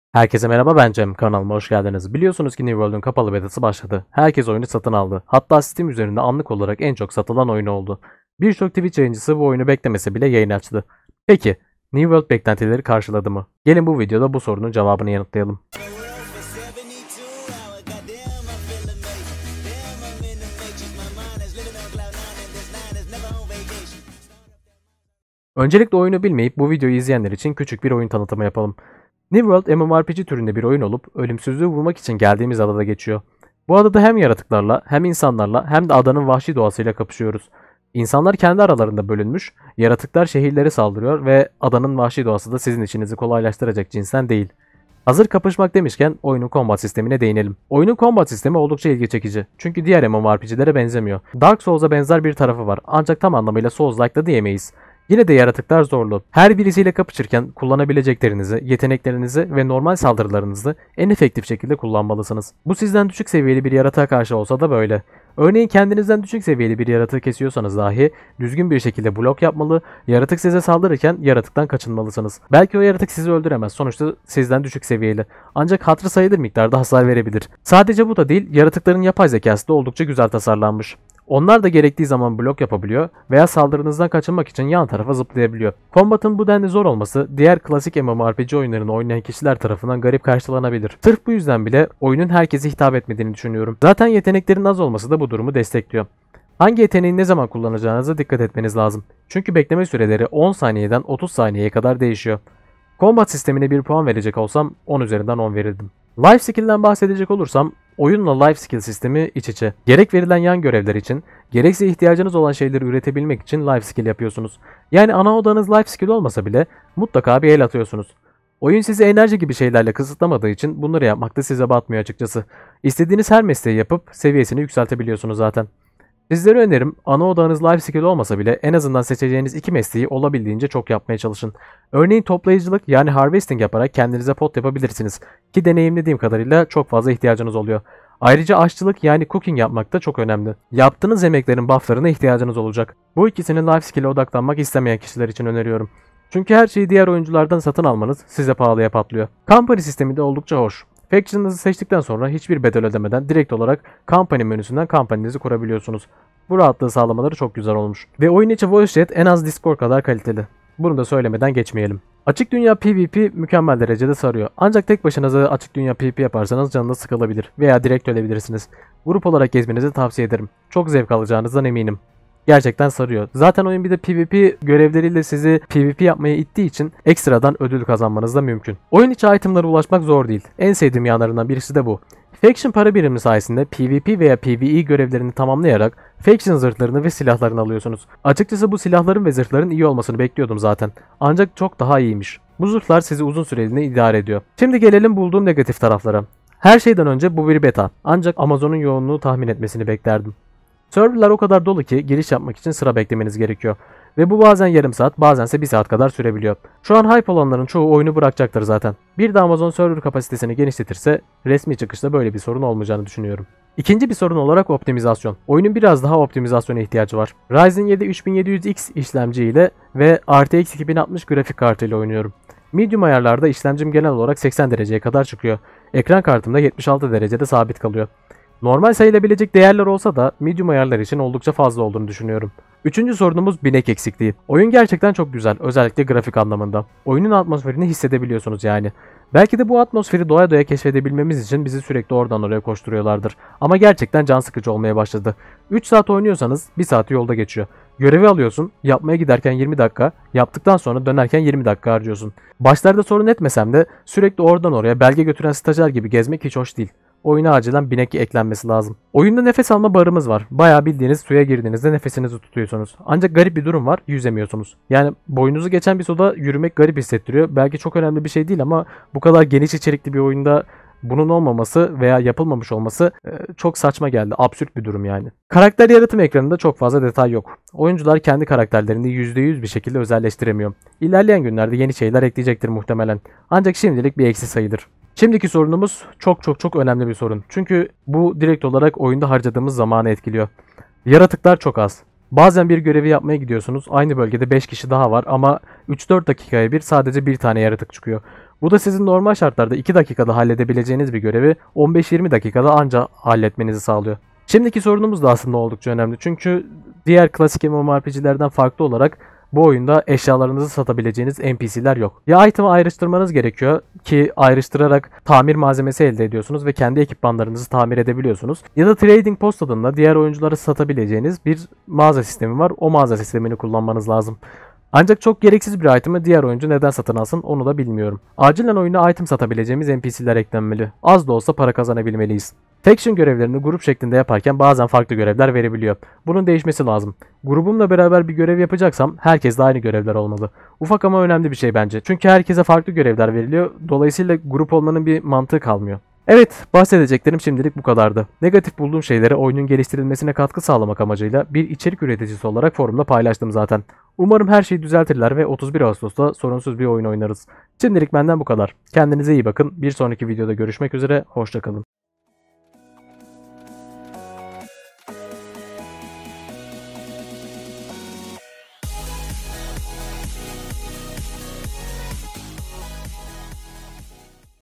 New World | Kapalı Beta Düşüncelerim
Herkese merhaba ben Cem, bugün kapalı beta hakkındaki düşüncelerimi sizlerle paylaşmak istedim.